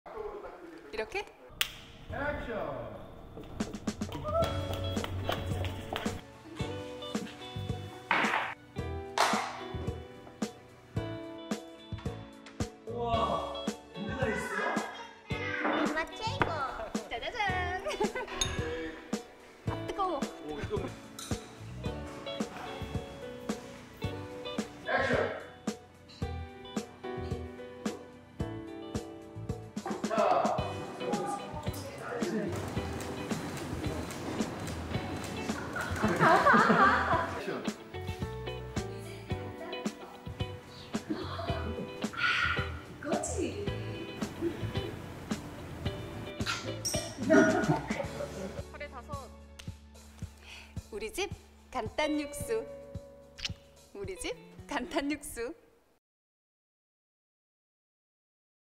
박솔미 간단요리사 TVCF 메이킹필름
2022년 박솔미 간단요리사 CF 메이킹필름 입니다.